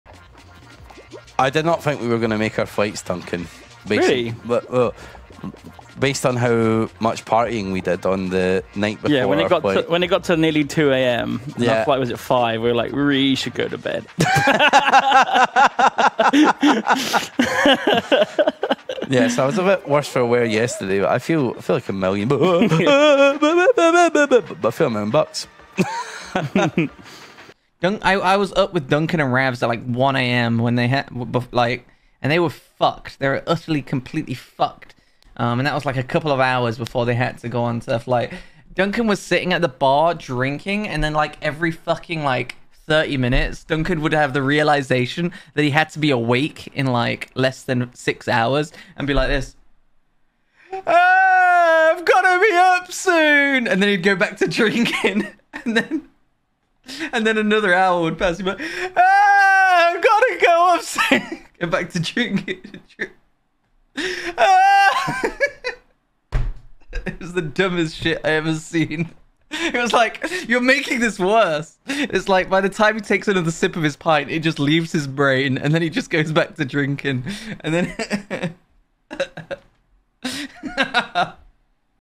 Why Duncan almost missed his flight
Duncan and Ravs almost missed their flight from TwitchCon.